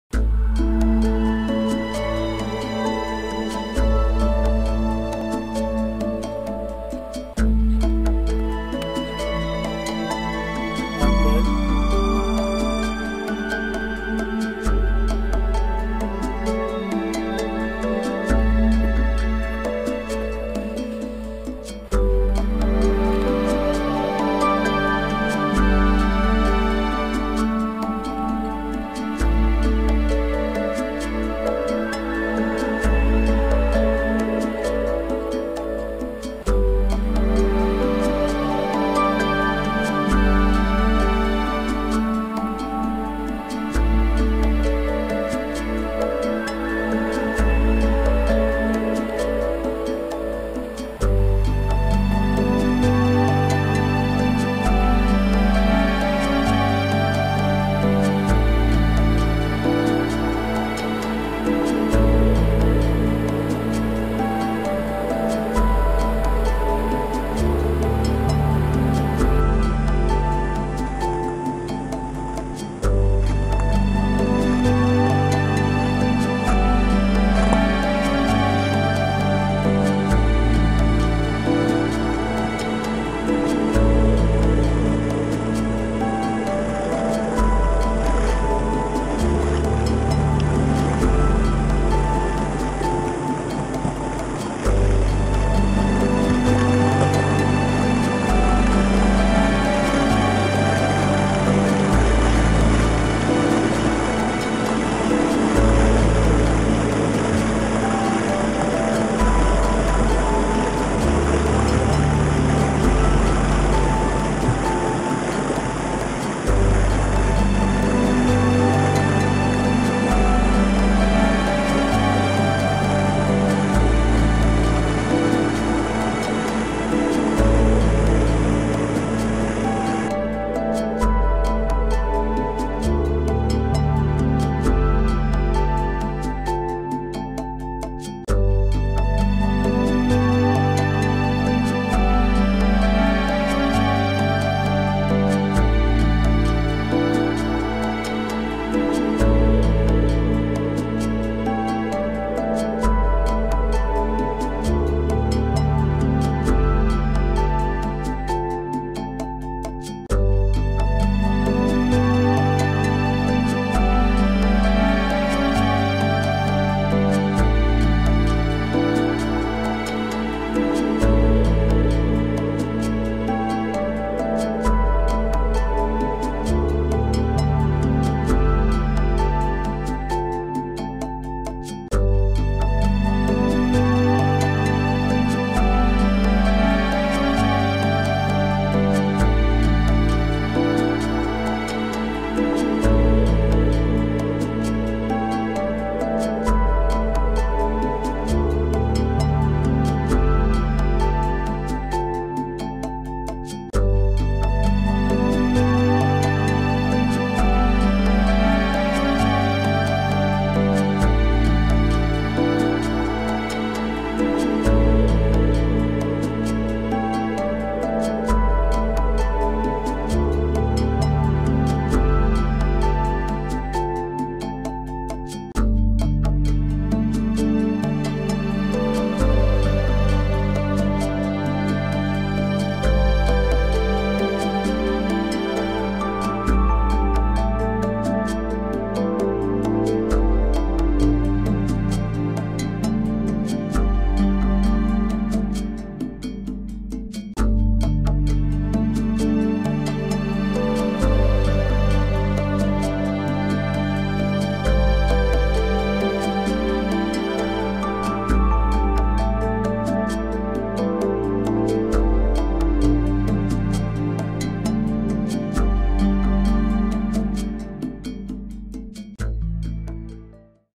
High Country May 2017
Testing the new REMOVU S1 during a recent trip to the high country.  Also, a short clip of Chris's fish from Rainbow Bridge.  A beautiful rainbow trout which sipped his custom dry fly pattern, drifted right inthebubbleline